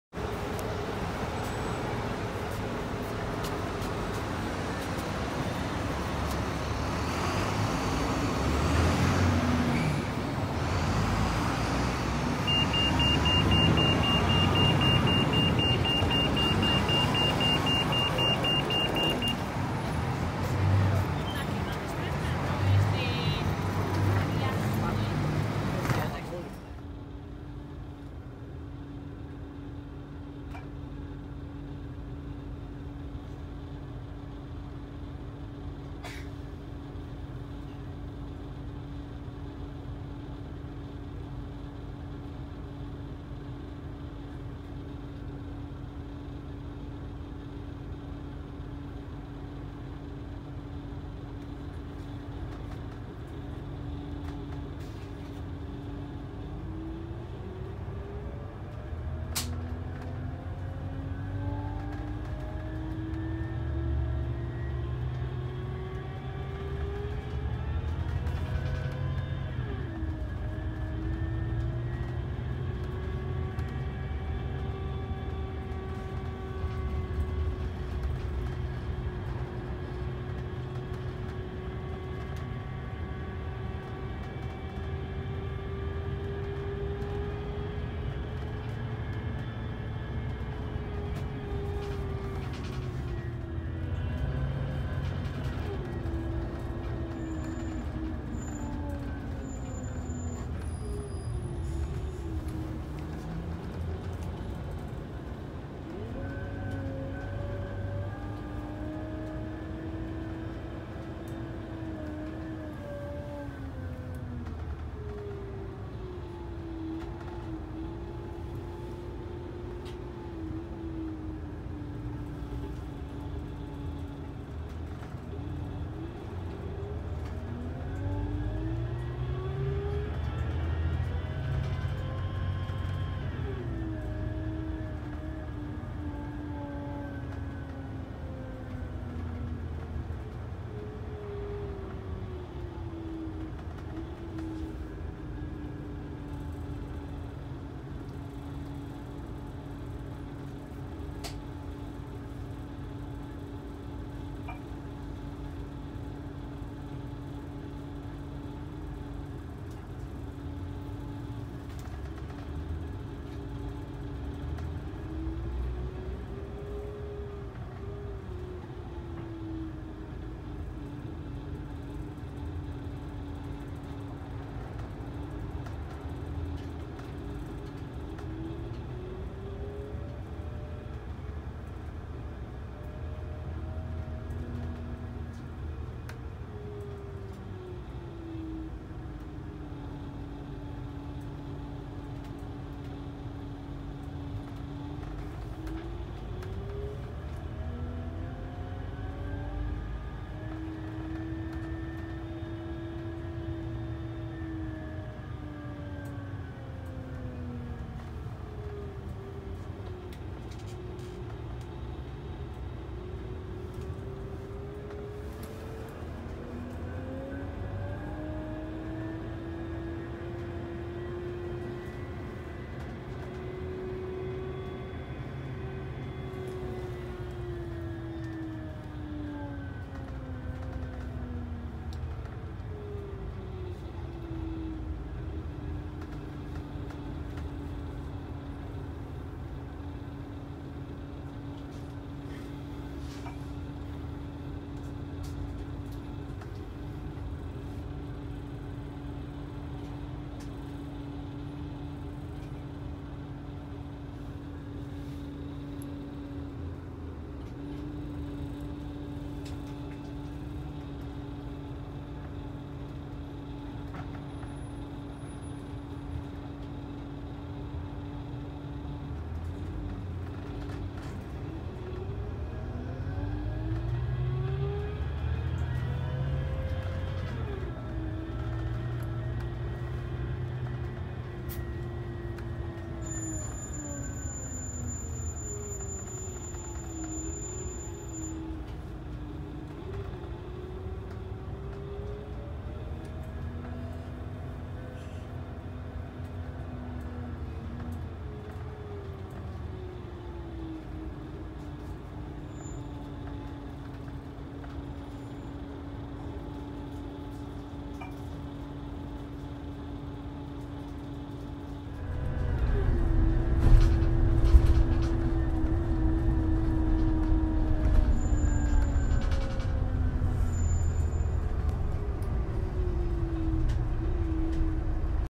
My Journey From London Victoria Station To Cambridge Via National Express 010
Here is my first edited video I made. These are some of the captures from my trip from London Victoria, to Cambridge, using national express service 010.